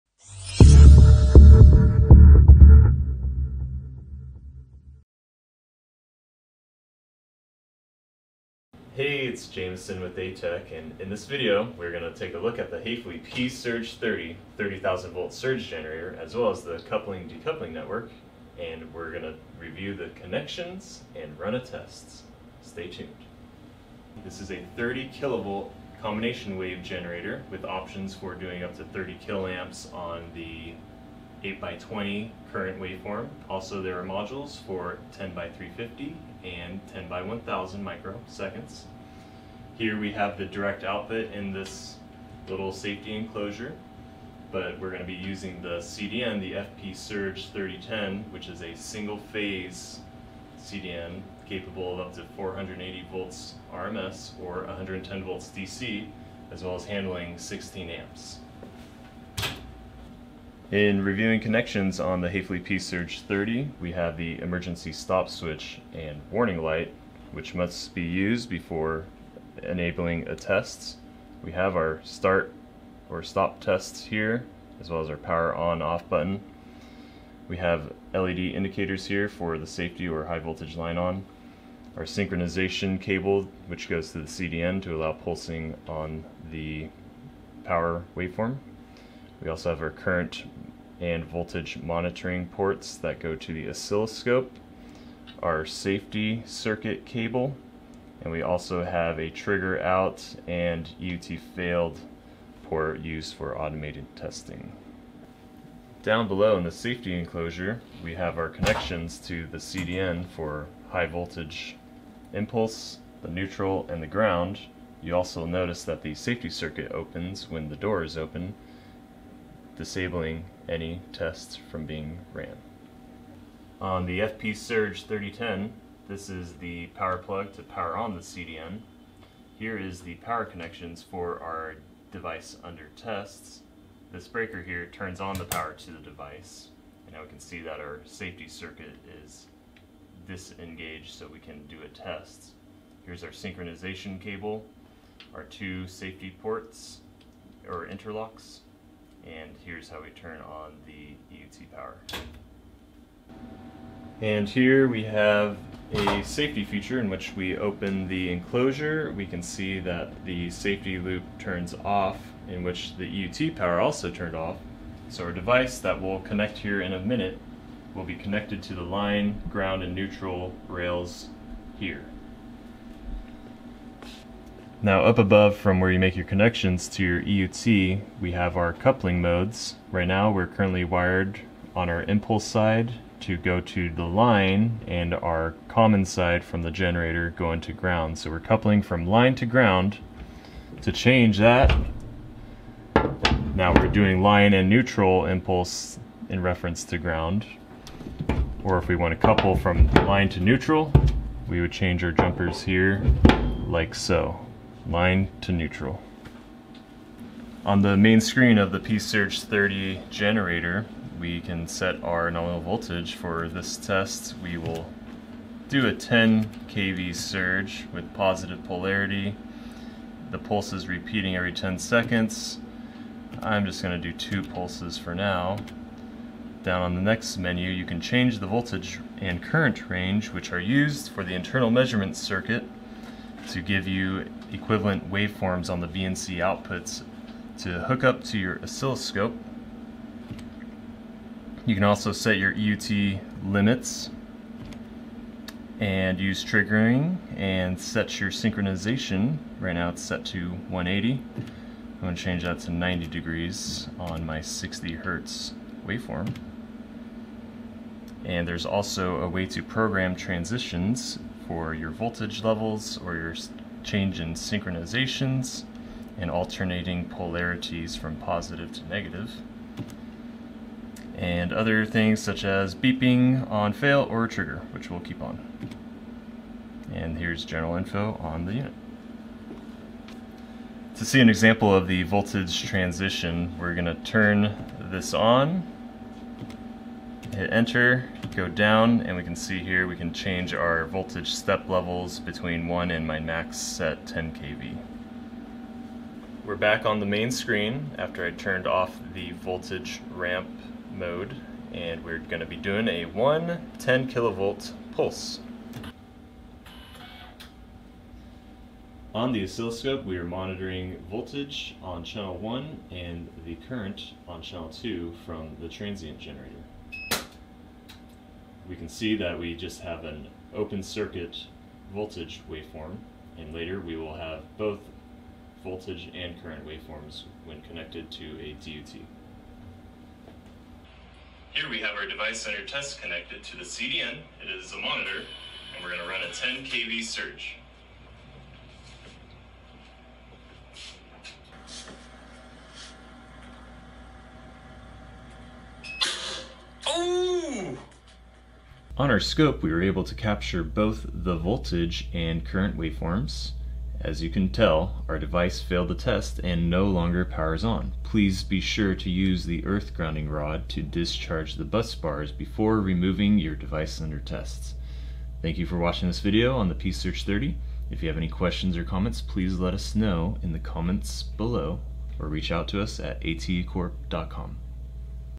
Haefely PSURGE 30.2 | 30kv Surge Generator for IEC 61000-4-5
In this video, we're excited to introduce you to the Haefely PSURGE 30.2 Surge Test System, a cutting-edge solution for precision surge testing and protection.

The PSURGE 30.2 is engineered to deliver reliable and accurate surge testing across a wide range of applications. Whether you're in the electronics, telecommunications, or automotive industry, this system empowers you to ensure the robustness of your equipment against power surges and transients.

Features:
- Up to 30kV impulse voltage
- Up to 30kA impulse current
- Automatic polarity switching
- Peak voltage and current monitoring
- Integrated large test cabinet
- Remote control option

Applications:
- Combination wave according IEC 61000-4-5
- Safety tests as defined e.g. in IEC 61010
- Telecom system tests according ITU K series
- Surge tests as described e.g. in IEC 60060-1 and ANSI C62.41
- Component test as described e.g. in IEC 60664 and IEC 61643-1
- Many IEC & EN Product standards

Join us as we dive deeper into the capabilities and benefits of the Haefely PSURGE 30.2 Surge Test System. Whether you're an engineer, researcher, or technician, this system is designed to simplify your surge testing process and ensure the reliability of your electronic devices.

Don't miss out on staying at the forefront of surge testing technology.